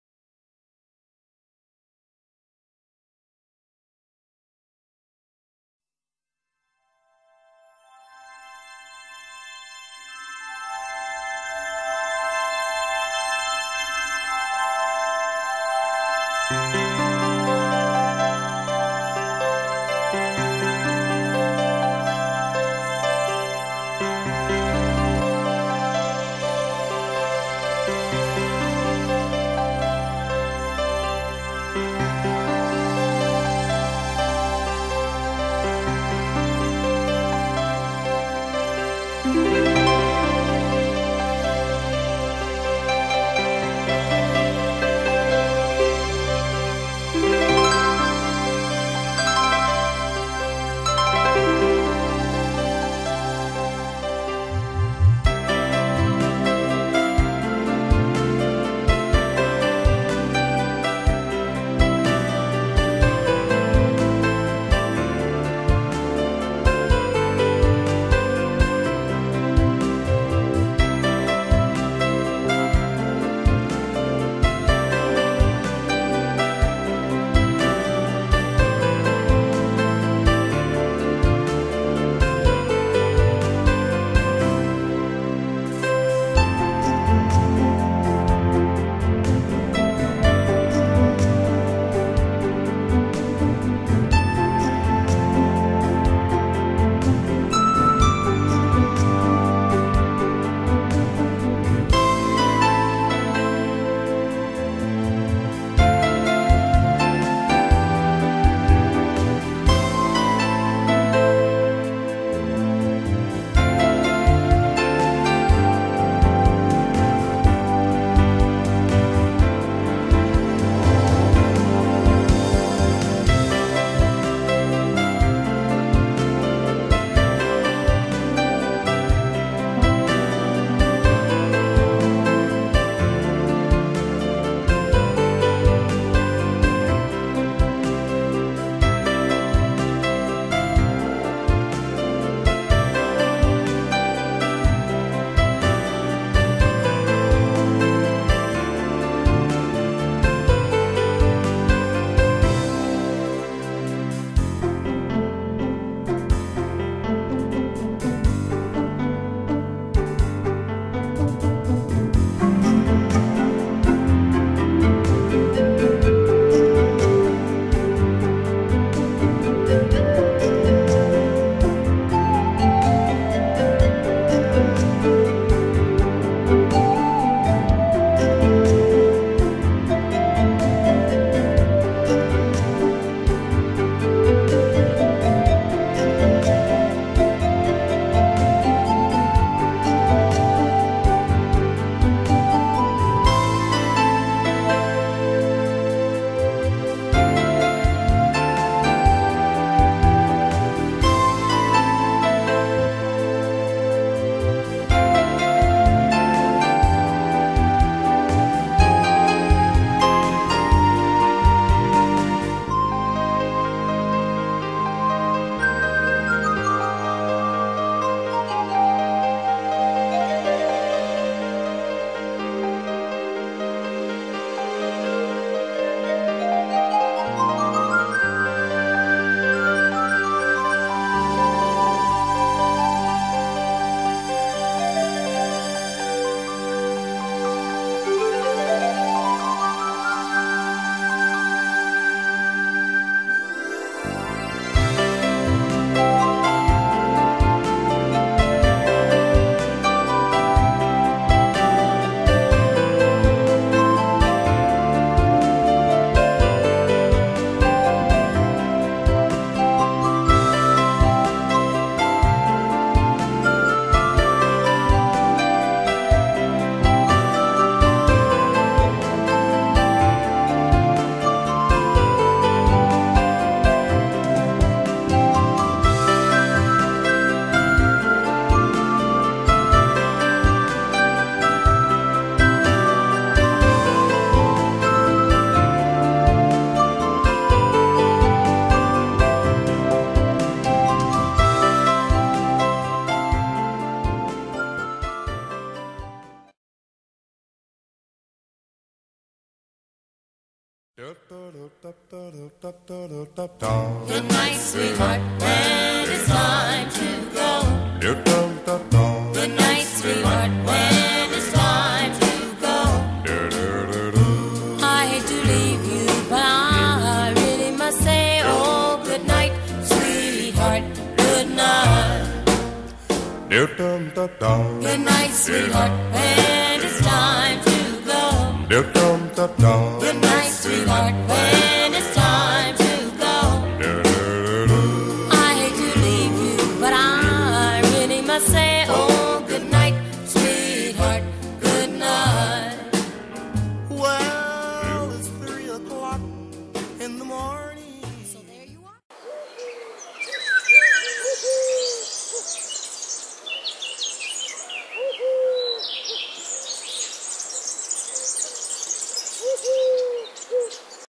Fairhope Alabama, My Back Yard, Spring 2016. Richard Stevens
My Fairhope, Alabama back yard this spring (2016) showing the many flowers and highlights.